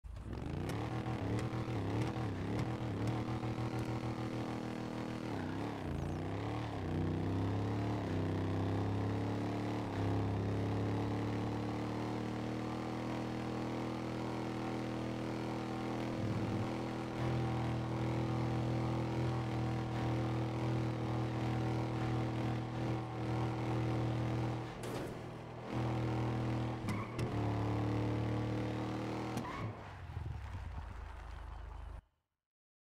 Chevy Malibu Donk 1978 | L11RY | Tuned
Top Speed is 400 mph, good turning, good brakes and acceleration.

Spawn Code: malibu